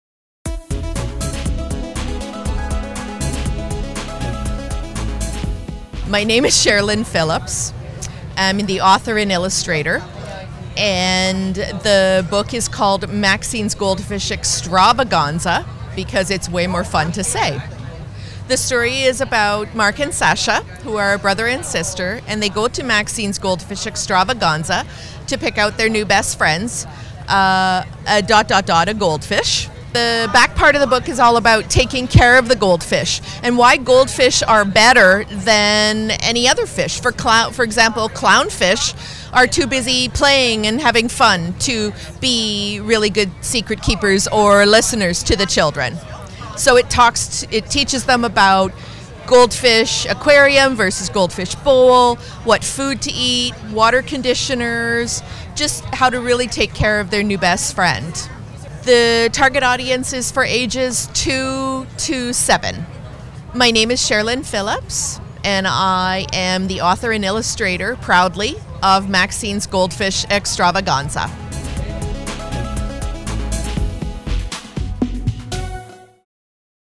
Sharelynne Phillips Maxine's Goldfish Extravaganza at WOS 2017
Author and illustrator Sharelynne Phillips ttalks about her kids' book at Word on the Street in Toronto